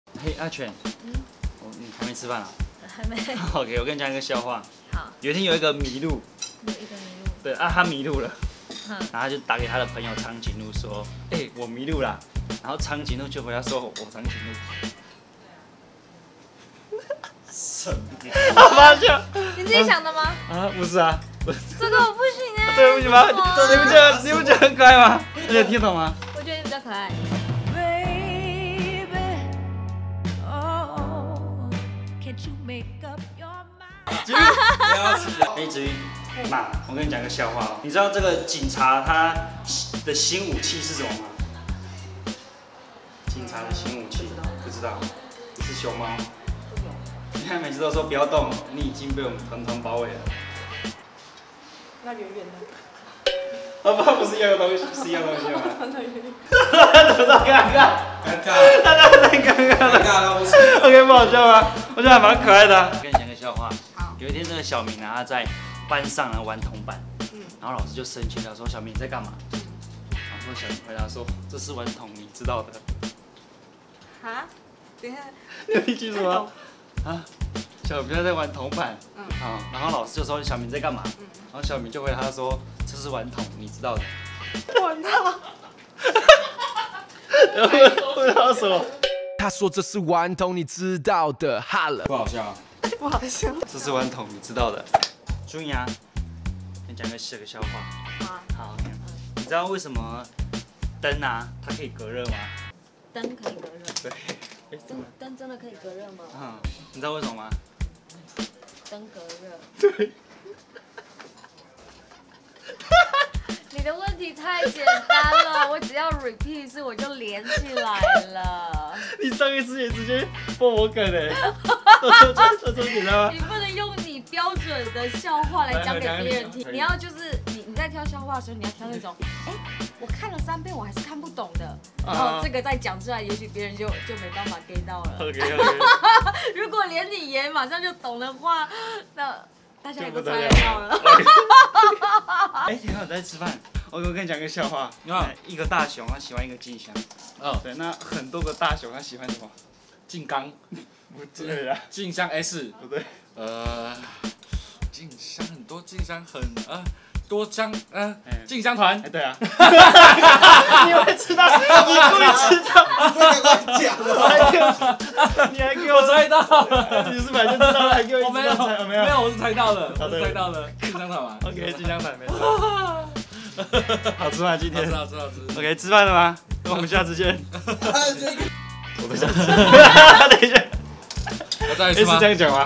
新冷笑話-笑話竟簡單到被連續破梗!?XDD
今天來分享幾個既可愛、又好懂得冷笑話XD
你們都有猜出來嗎 哈哈哈

PS:如果有想提供笑話給我的，歡迎底下留言or私訊，被我挑中不錯的笑話。會送你一個精美小禮物哦^^

官方LINE「@fnx3248L」